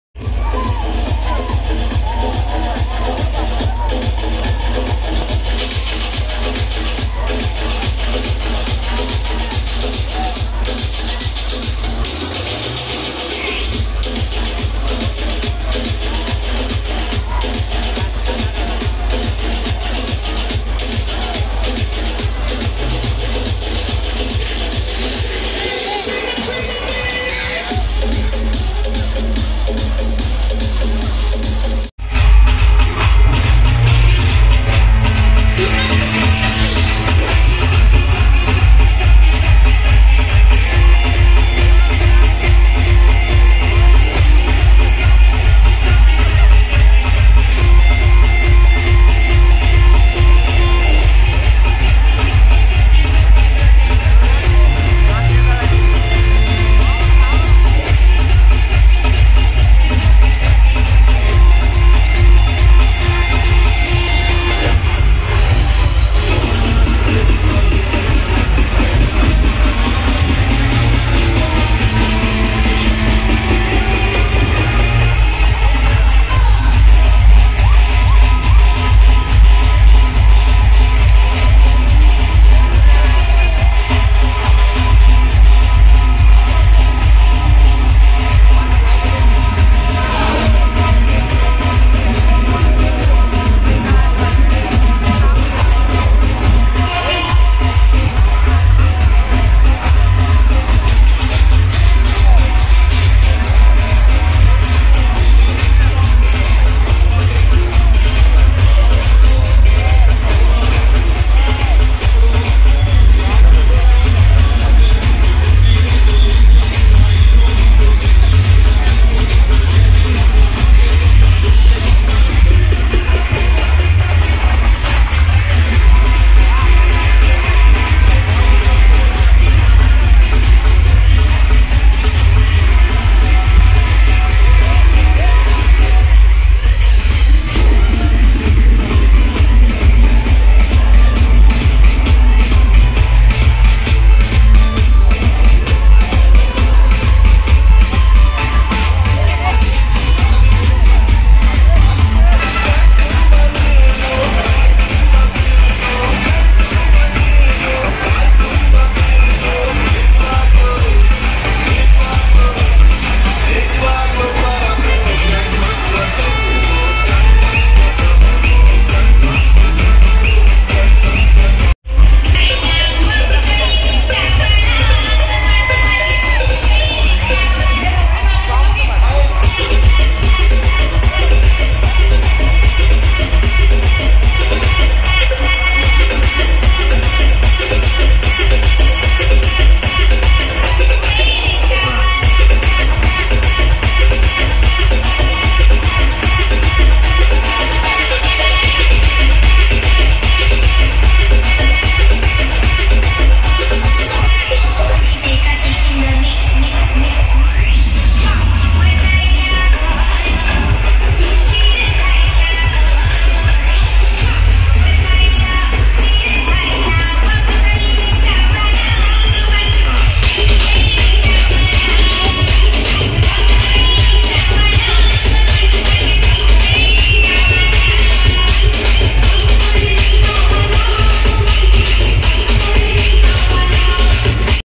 Krish Standard Sound | Ganesh Visarjan 11th day Hubli | Hubli blast 💥💣
HEAVY CROWD 🤯🤯🥳
❌HUBLI : THE CHOTAMUMBAI ❌